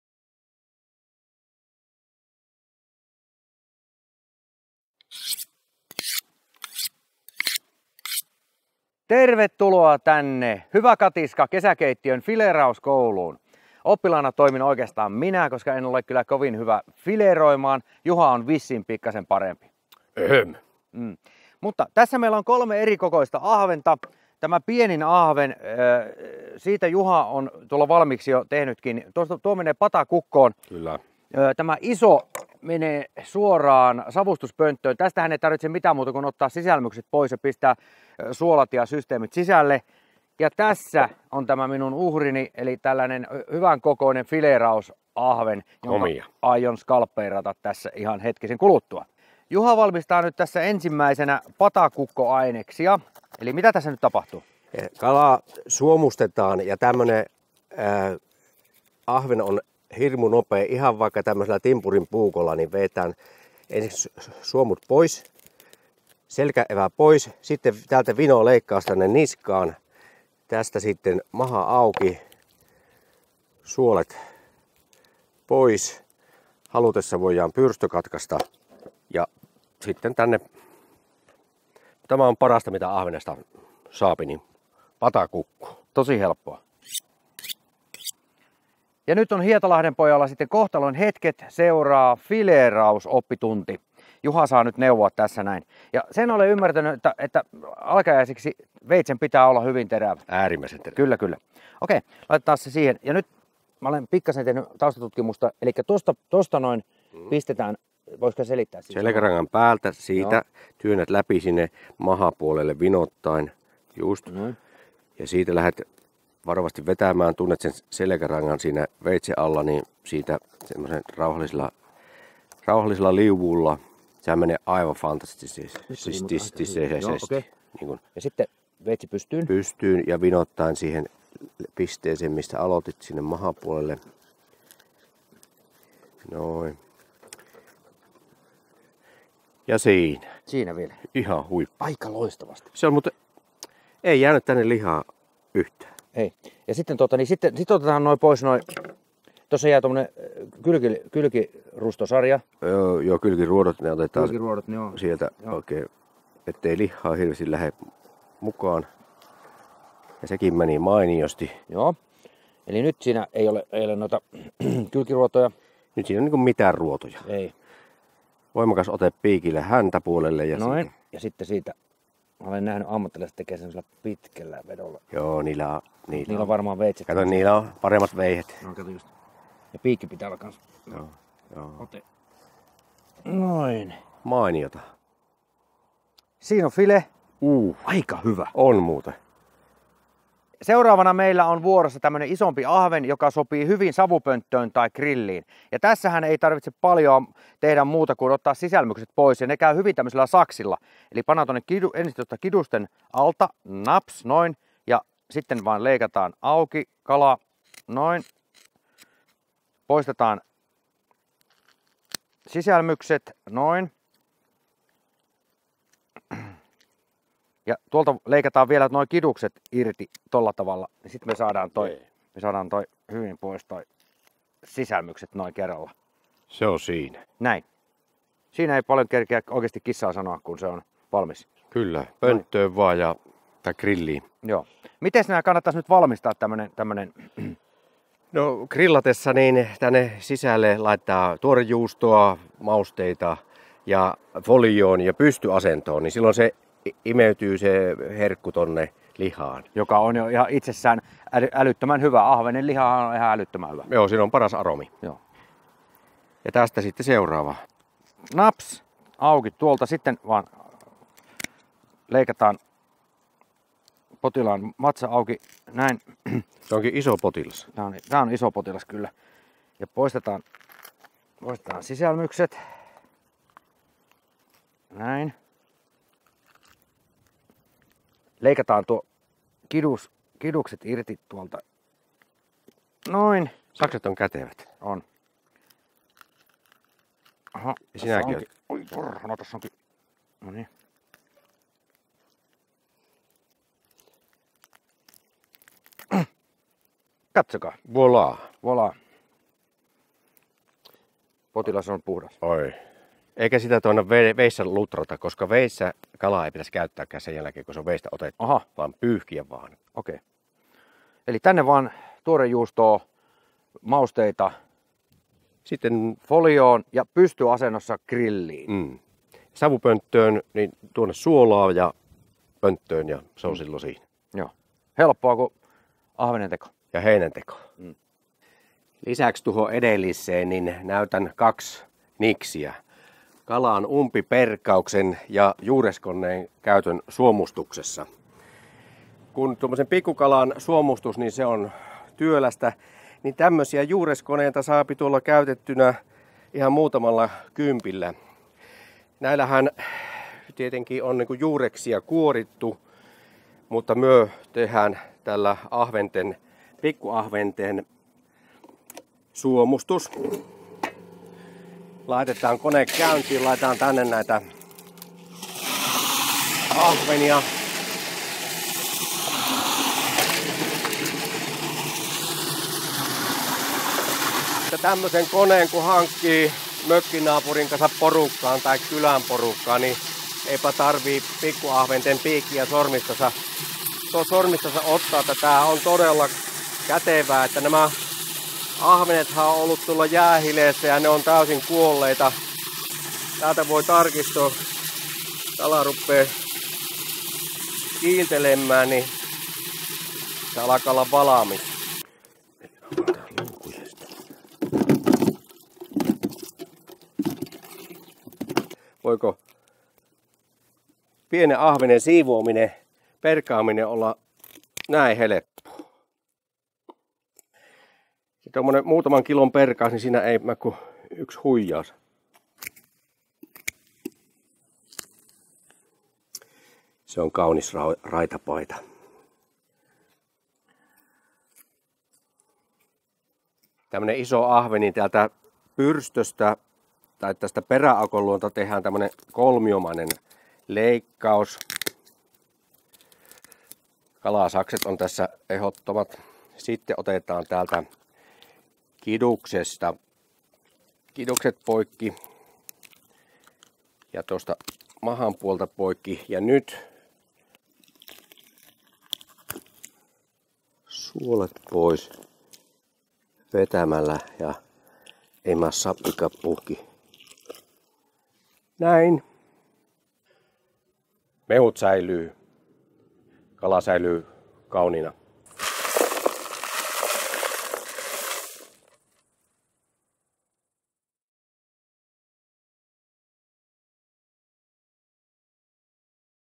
Hyvä Katiska, Kalankäsittely
Hissu ja Juha perkaavat kaloja